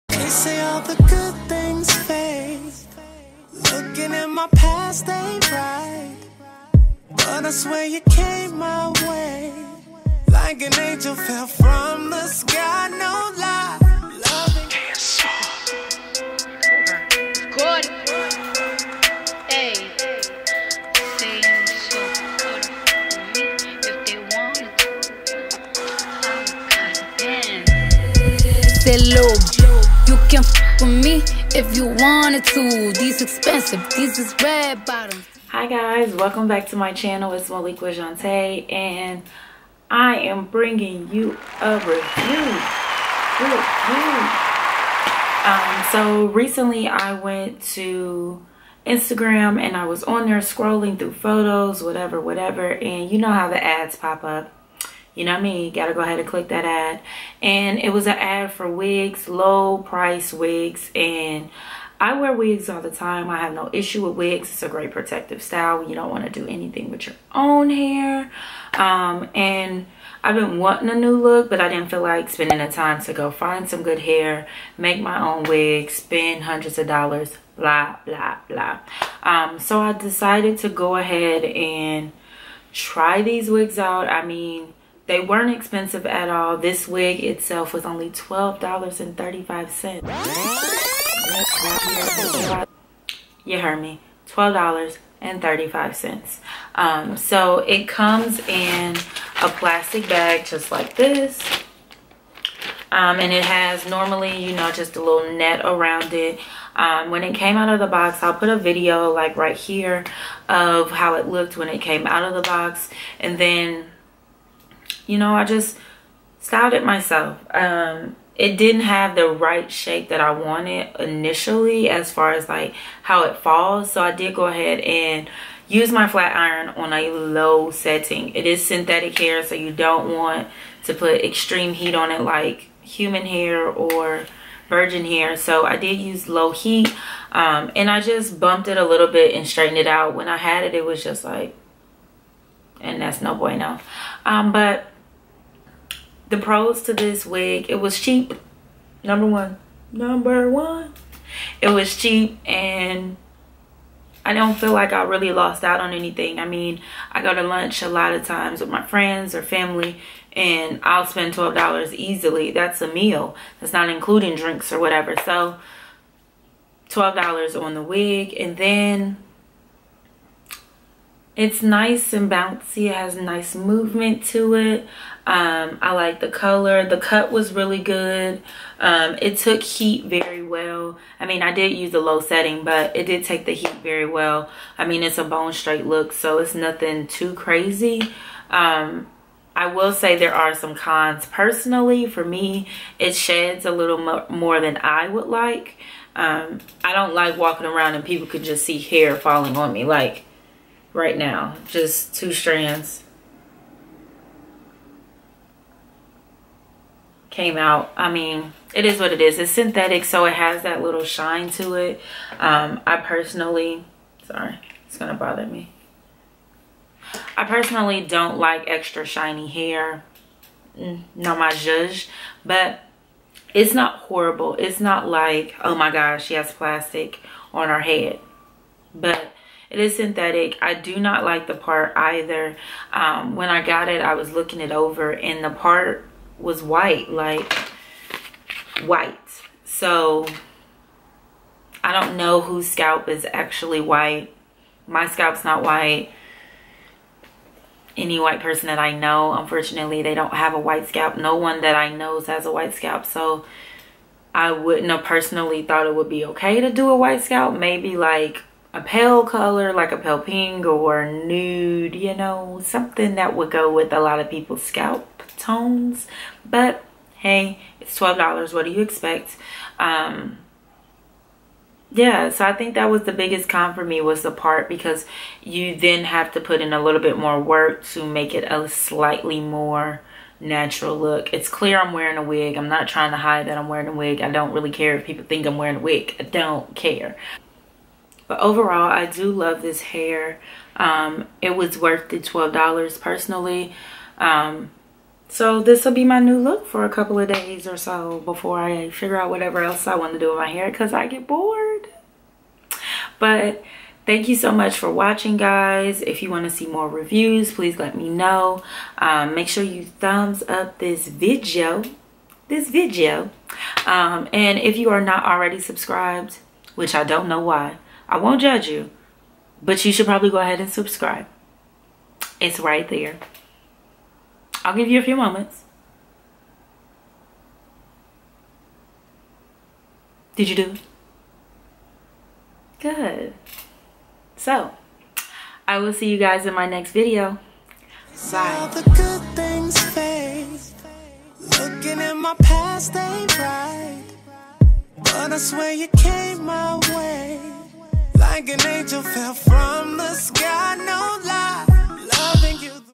Bomb Wig ONLY $12.35!!!! 😳😳😳DressLily Wig Review!
Hey Dolls Below are the details;

Wig Specs:
Adiors Medium Straight Bob
Center Part
Synthetic Wig
14inches
Color: Black